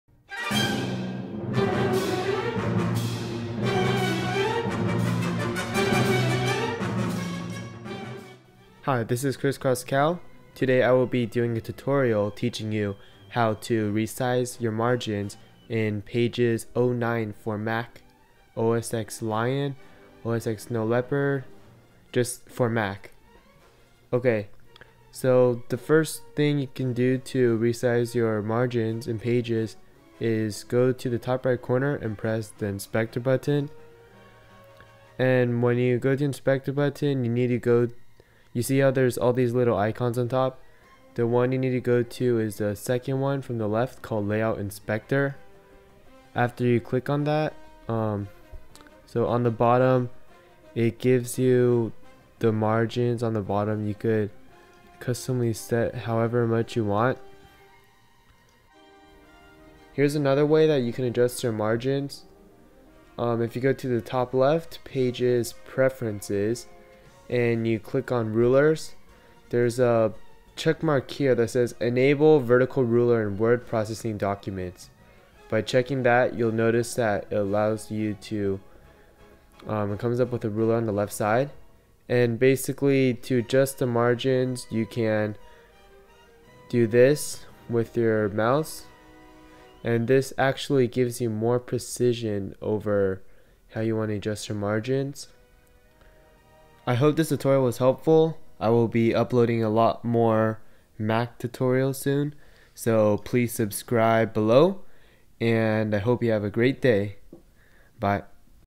How to Change Margins in Mac Pages '09
This is a tutorial teaching you how to change your margins in Pages '09 for Mac. Great for students having trouble resizing to 1 inch margins for MLA format. I used to have this same problem, and I want to share with everyone how to work around a simple problem.